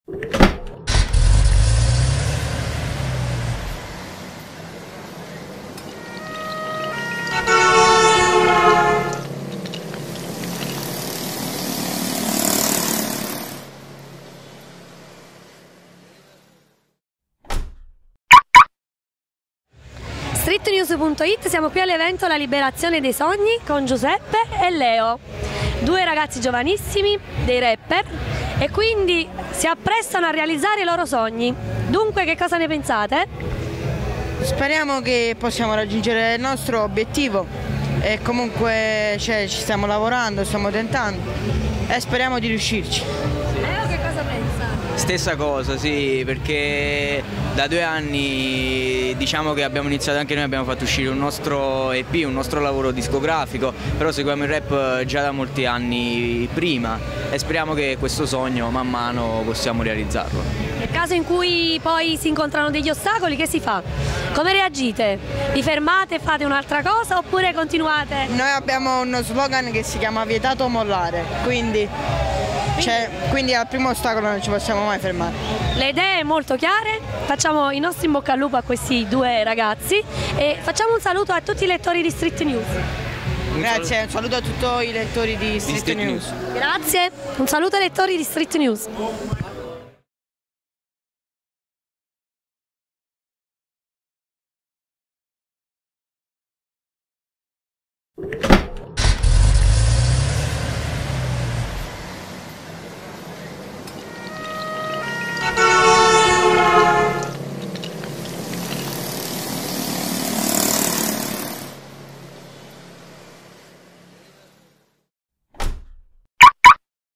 StreetNews.it-G Crym-9°Anniversario Hollywood Animation
In occasione del 9° Anniversario Hollywood Animation di Cimitile (Na) del 1 Maggio 2016,  l'inviata Margherita Saporito intervista i rapper  Giuseppe e Leo (G Crym).

Per info e contatti:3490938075 (Davide Napoletano)

Intervistatrice: Margherita Saporito
Rapper: G Crym
Fondatore e Direttore di StreetNews.it: Davide Napoletano
Photographer/Video e Montaggio : Sannino C.M.G Photographer